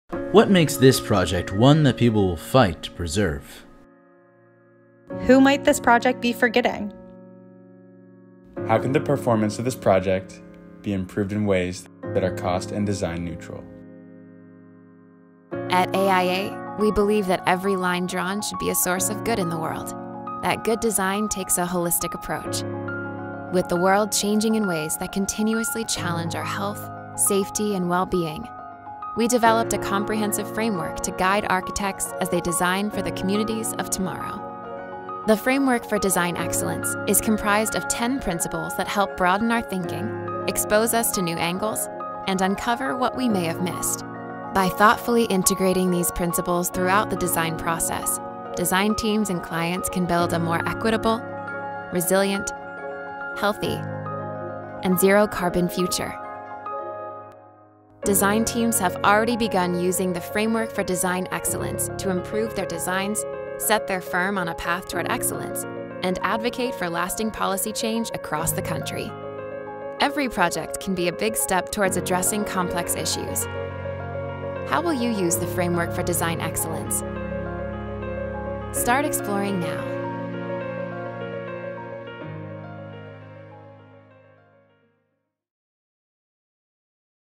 The AIA Framework for Design Excellence
The Framework for Design Excellence represents the defining principles of good design in the 21st century. Comprised of 10 principles and accompanied by searching questions, the Framework seeks to inform progress toward a zero-carbon, equitable, resilient, and healthy built environment. How will your firm integrate the Framework principles into your work?